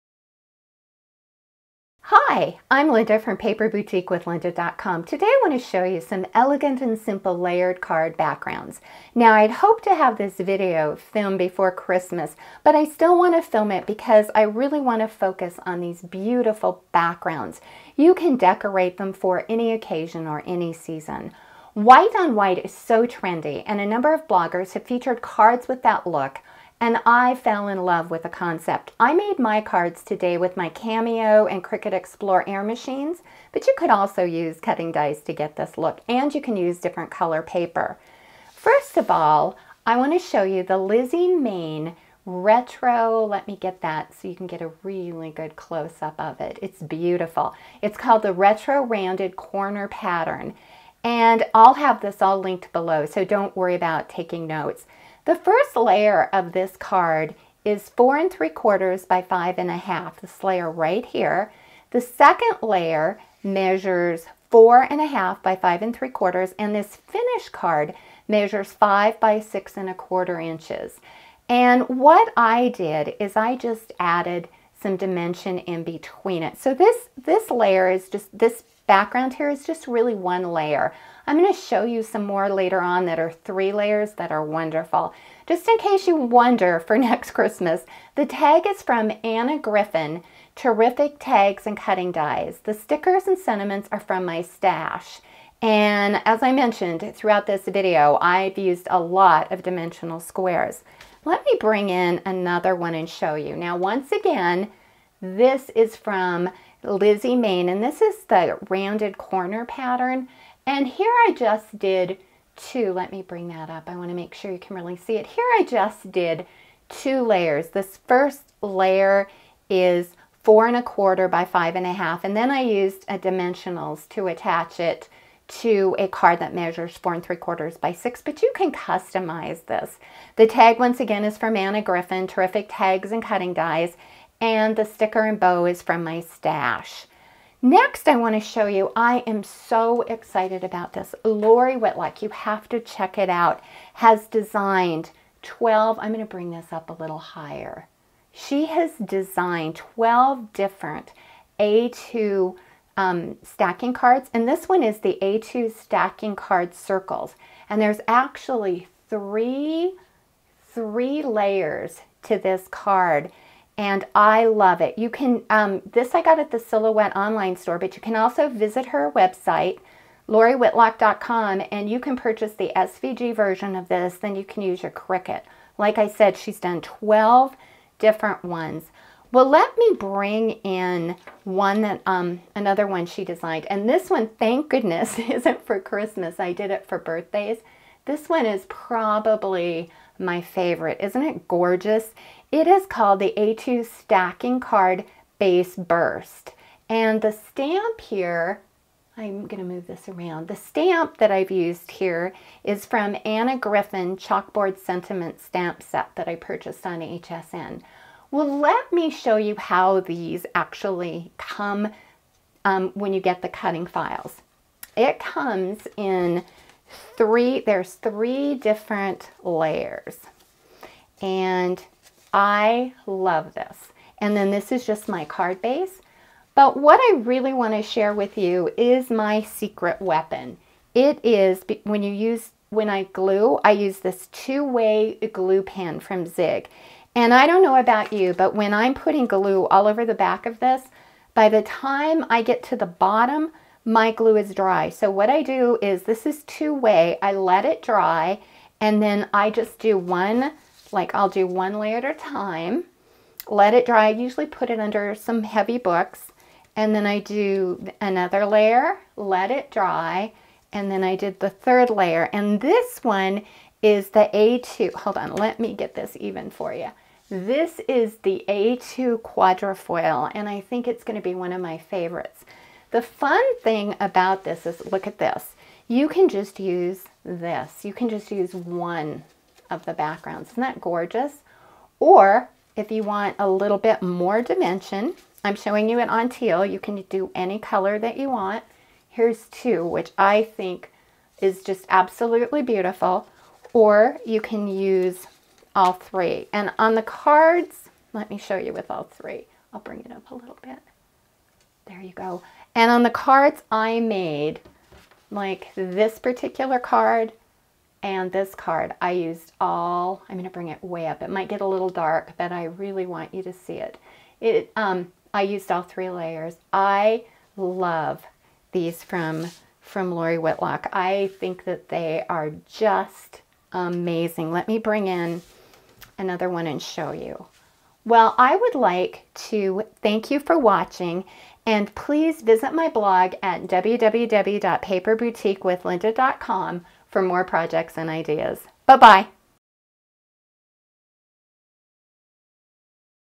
Layered Card Backgrounds
In this video I demo how to create layered card back grounds using white on white paper.
Below are the links mentioned in my video:
Lizzie Mayne Retro Rounded Corner Pattern: Purchased from Silhouette On-line Store #51112.
A2 Stacking Card Base Circles #165922 designed by Lori Whitlock: Purchased from Silhouette On-line Store. Also available in SVG format and can be cut with Cricut Explore
A2 Stacking Card Base Burst #165913 designed by Lori Whitlock: Purchased from Silhouette On-line Store. Also available in SVG format and can be cut with Cricut Explore
A2 Stacking Card Base Quatrefoil #165925 designed by Lori Whitlock: Purchased from Silhouette On-line Store. Also available in SVG format and can be cut with Cricut Explore
Zig Memory System Two Way Glue Pen Fine Tip